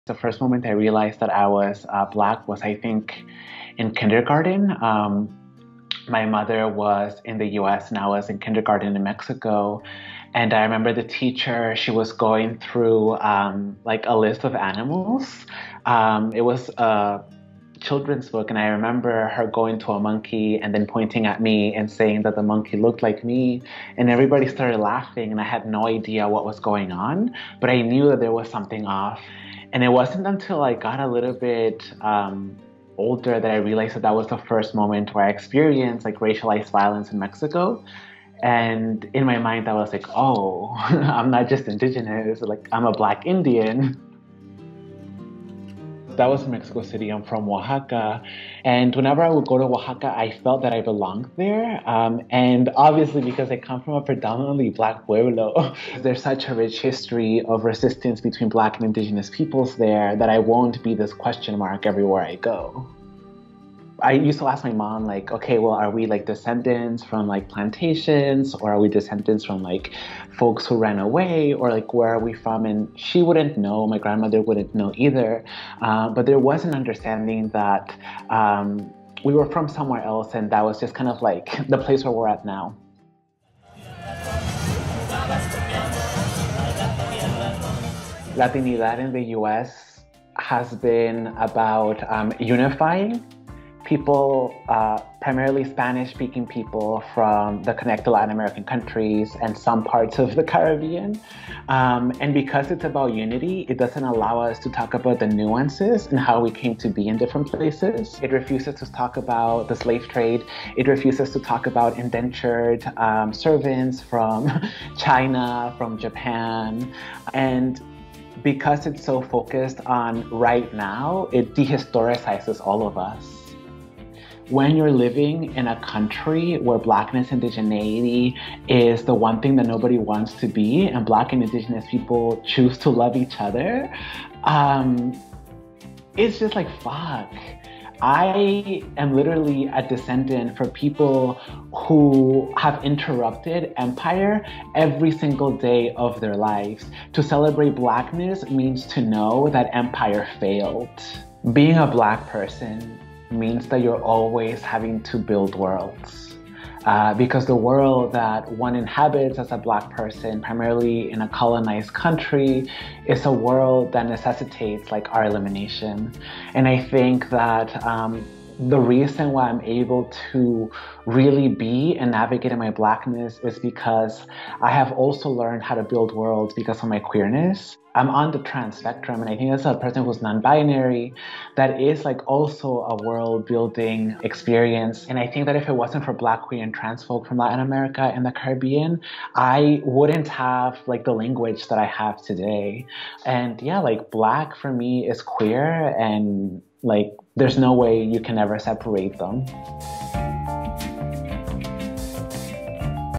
Afro-indigenous Poet Alán Pelaez Lopez Breaks Down Why Blackness Is Radical in Latinidad
"To celebrate Blackness means to know that empire failed."

Afro-Indigenous poet and cultural worker Alán Pelaez Lopez says that centering Blackness in an AfroLatinx framework is both beautiful and radical